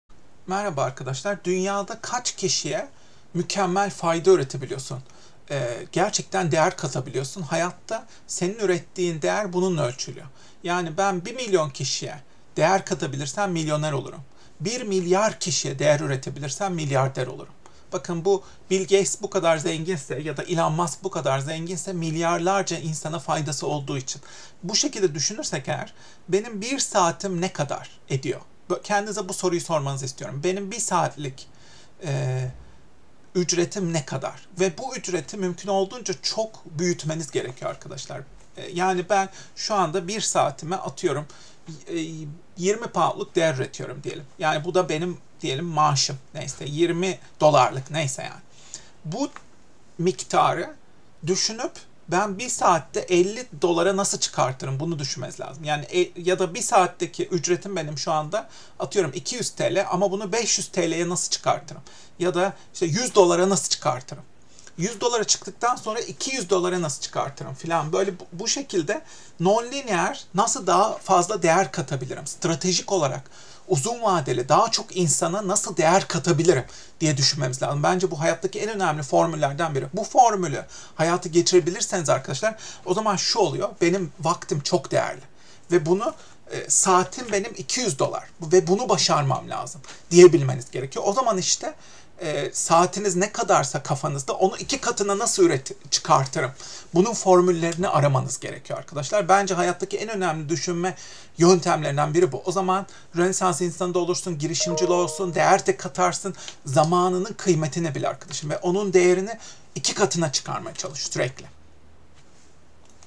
Sizin 1 Saatlik Ücretiniz Ne Kadar? Peki Siz 1 Saatte Ne Kadar Değer Üretiyorsunuz?
Dünyada kaç kişiye fayda üretebiliyorsunuz ve değer katabiliyorsunuz?

Eğer 1 milyon kişiye ulaşarak değer katabilirseniz, milyoner olursunuz.

Eğer Elon Musk veya Bill Gates gibi 1 milyardan fazla insana ulaşarak değer katabilirseniz, milyarder olursunuz.

Mesele pastayı büyütmek, değer katmak, diğer insanların hayatını pozitif şekilde değiştirmek ve dönüşürmek.

Şu anda aylık maaşınız kaç? Peki 1 günlük mesainizden kaç para kazanıyorsunuz?  Peki 1 saatinizden ne kadar kazanıyorsunuz?

Bunu hesaplayın.

Sonra 1 saatlik ücretinizi nasıl 2 katına çıkarabilirsiniz, bunu düşünün.

*Daha fazla insana fayda üretebilir misiniz?
*Ürün veya içerik çıkarıp Internette paylaşabilir misiniz?
*Daha akıllıca ve stratejik olarak çalışıp daha fazla değer katabilir misiniz?

Her yıl saatlik ücretinizi 2 katına çıkarmak hedefiniz olmalı.

Mesela, şu anda saatlik ücretim 5 dolar diyelim.  Seneye 10 dolar olmalı. 2 sene sonra 20 dolar olmalı. 3 sene sonra 40 dolar, 4 sene sonra 80 dolar seviyesine çıkmaya çalışmalısınız.

Yani böyle non-lineer ve logaritmik olarak ilerlemelisiniz.

Bu da ancak radikal düşünerek, inovasyon üreterek, girişimci gibi düşünerek, risk alarak, sürekli öğrenerek, yüzlerce binlerce deney yaparak olur.

İşte bunu başarmalısınız!